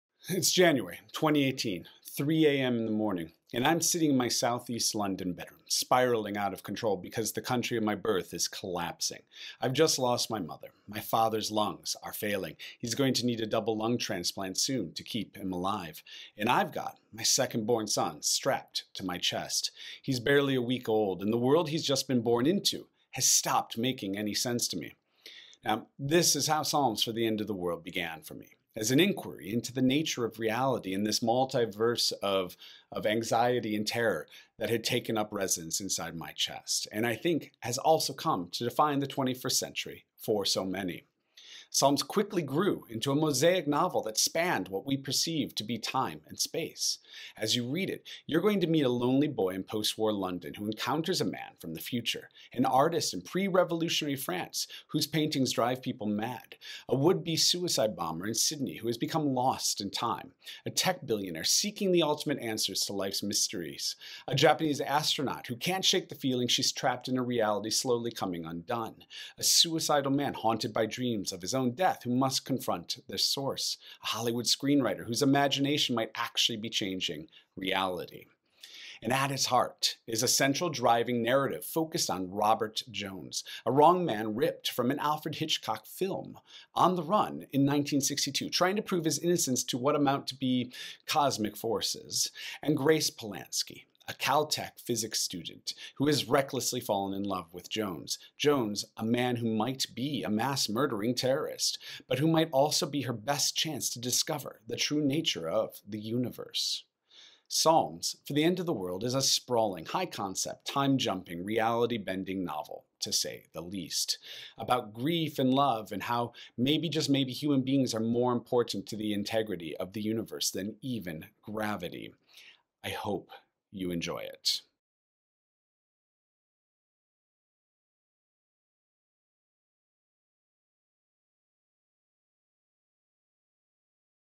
PSALMS FOR THE END OF THE WORLD - an introduction from author Cole Haddon
'A book designed to be more than the sum of its parts, and one that achieves that because love is the thing that binds it together. Vitally fresh' DOMINIC NOLAN

'A trans-dimensional, kaleidoscopic mystery-box of a novel. wholly and riotously original. Haddon is a mad scientist of genre and his epic is a tour de force' PETER NO DAVIES